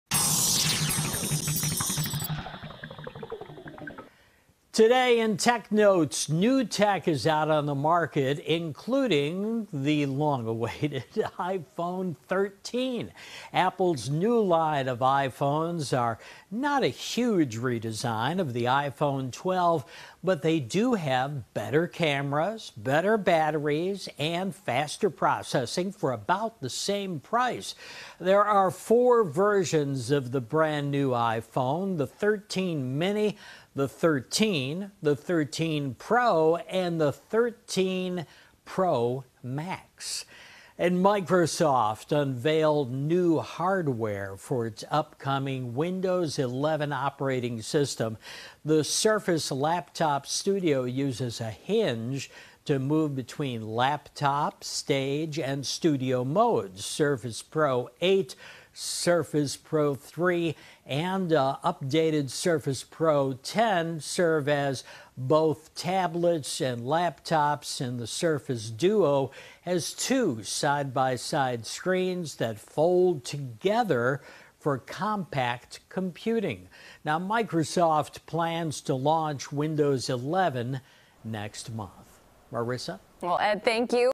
Ed's Tech Notes: New iPhone 13 Goes On Sale, Microsoft Unveils New Technology
Apple’s new iPhone 13 line is not a big redesign of the iPhone 12, but the new phones do have better cameras, batteries, and processing. Meanwhile, Microsoft unveiled new hardware for its upcoming Windows 11 operating system. CBS 2’s Ed Curran breaks it down.